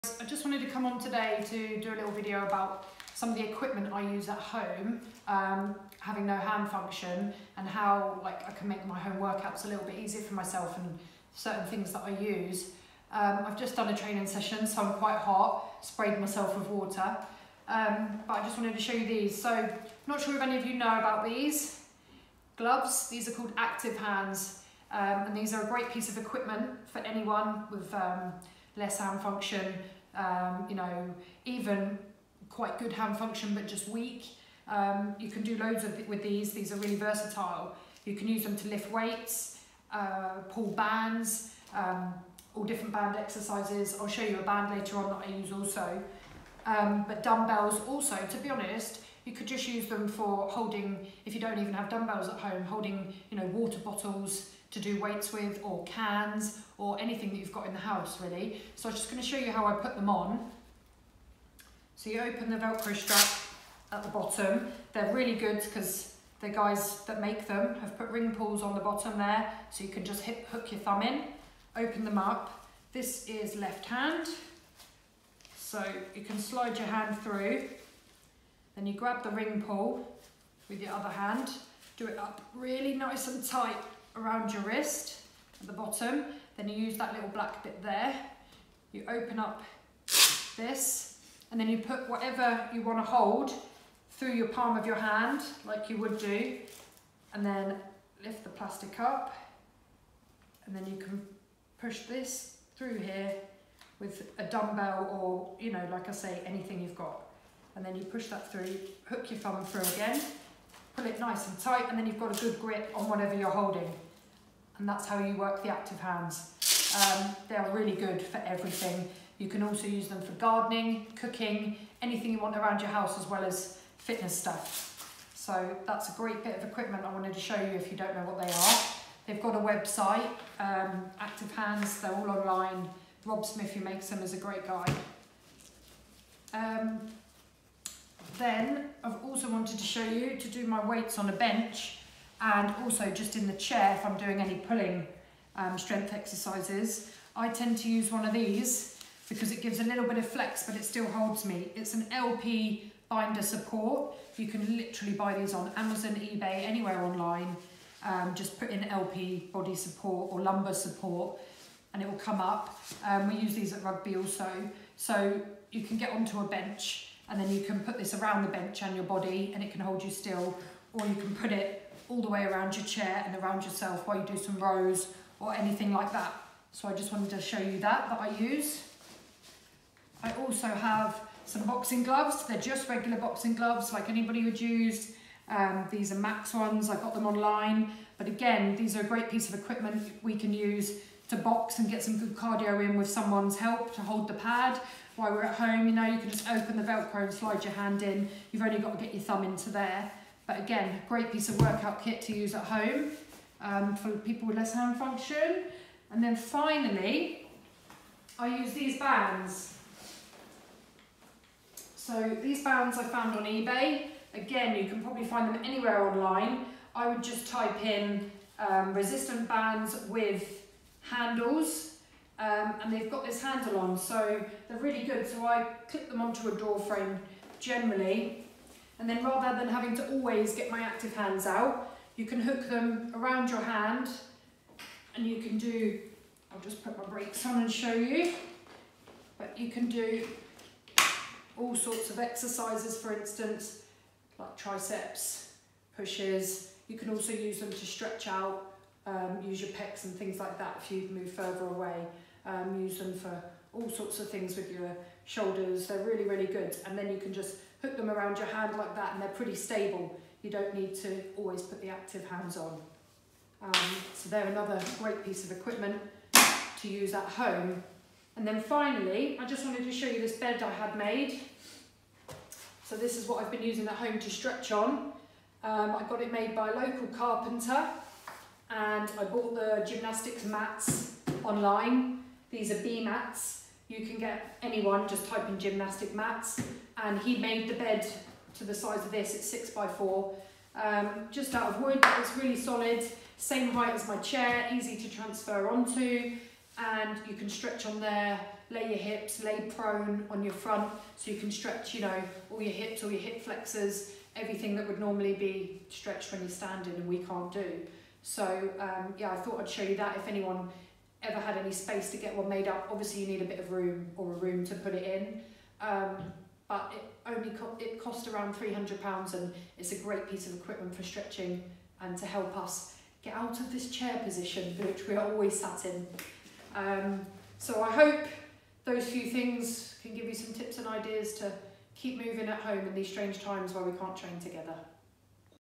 Kylie Grimes - Workout Equipment Guide
Check out this workout equipment guide from our new ambassador and GB Wheelchair Rugby star Kylie Grimes!